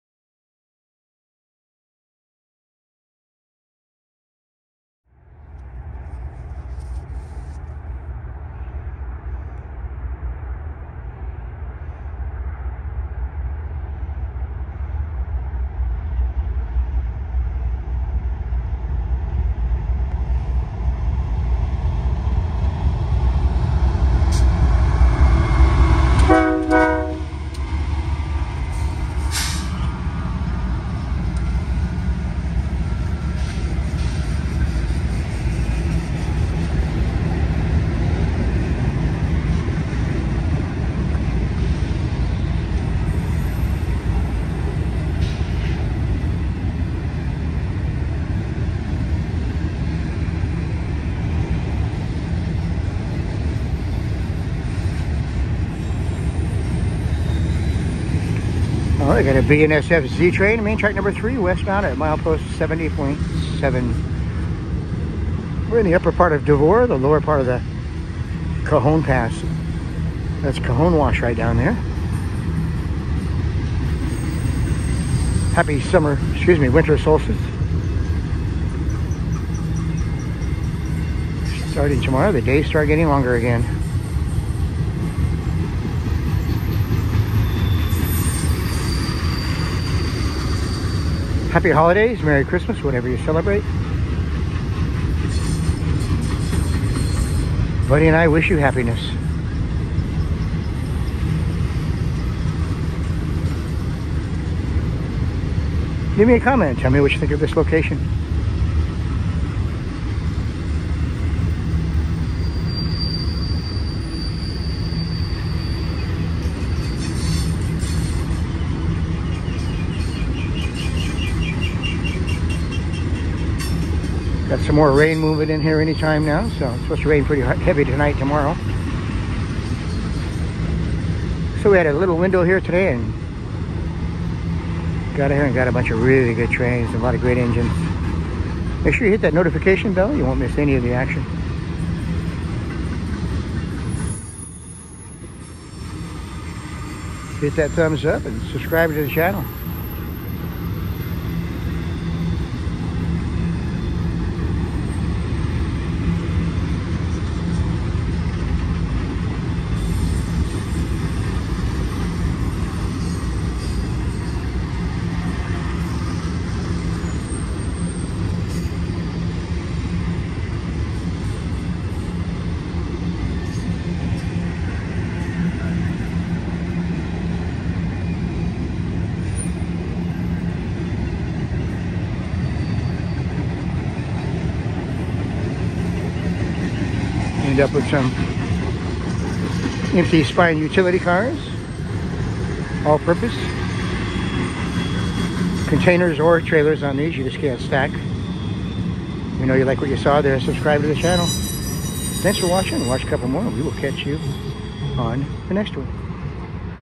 UPS - North Bay, CA to Hobart Yard, Los Angeles, CA. WB Z Train Fed Ex UPS & Baretables Utility Cars
UPS - North Bay, CA to Hobart Yard, Los Angeles, CA. WB Z Train Fed Ex UPS & Baretables Utility Cars. At Devore on the BNSF Cajon Subdivision.